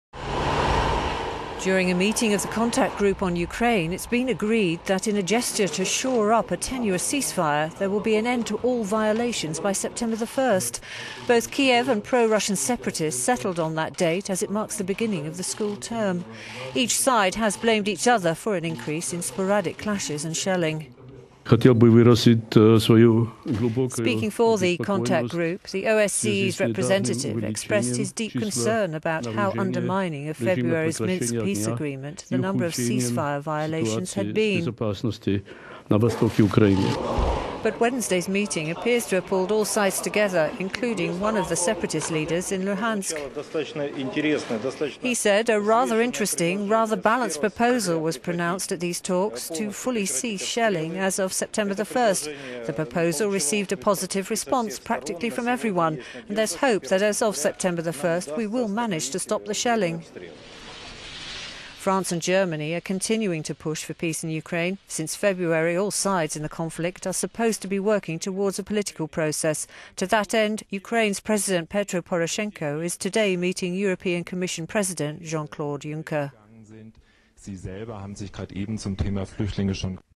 Ukraine and separatists agree next week as deadline for truce
During a meeting of the Contact group on Ukraine its been agreed that in a gesture to  shore up a tenuous ceasefire, there will be an end to all violations by September 1.

Both Kyiv and pro Russian separatists settled on that date as it marks the beginning of the school term.

Each side has blamed the other for an increase in sporadic clashes and shelling.

Speaking for the Contact group, the OSCE's representative Martin Sajdik expressed his deep concern about how undermining of February's Mins…